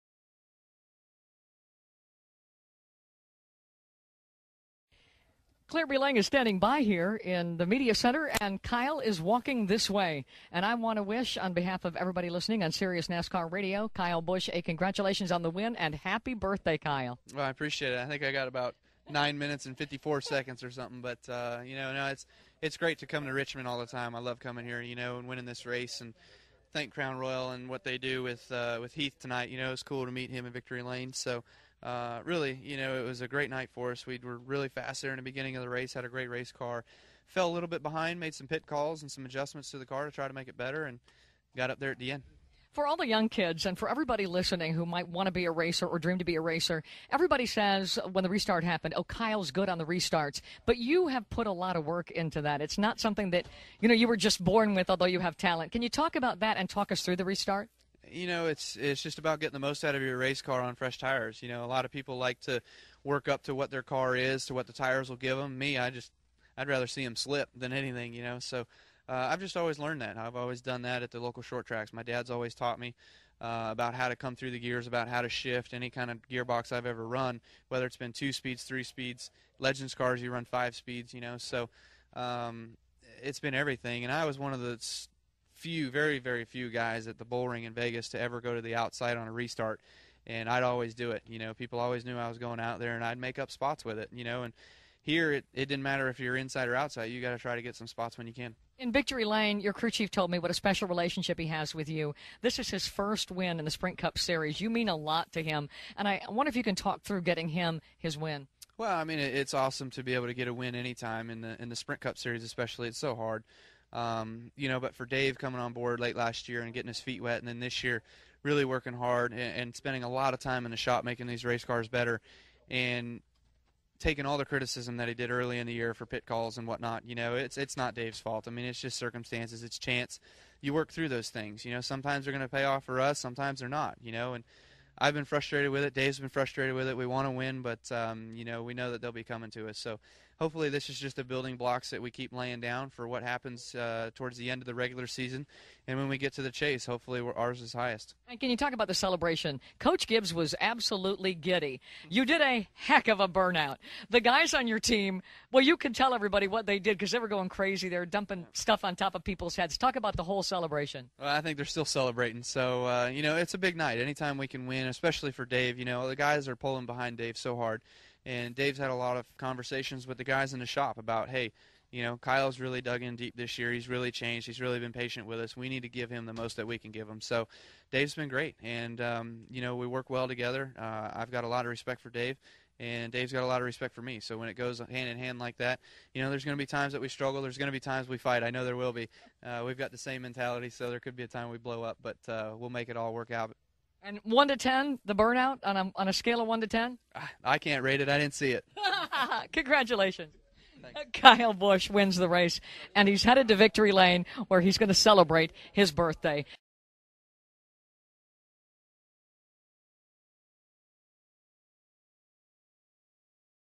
What Makes Richmond Winner Kyle Busch So Good on Restarts? // SiriusXM // NASCAR Radio
Your radio home for 24/7/365, unprecedented inside access to everything NASCAR.

Kyle Busch took advantage of a late restart to get to Victory Lane in Richmond on Saturday, May 1. After the win, he spoke with SIRIUS NASCAR Radio's Claire B. Lang.